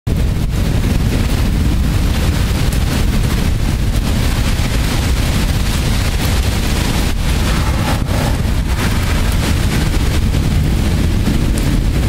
Severe Weather August 26th 2024
70 mile an hour winds, heavy rain, and hail. Severe weather warnings in effect.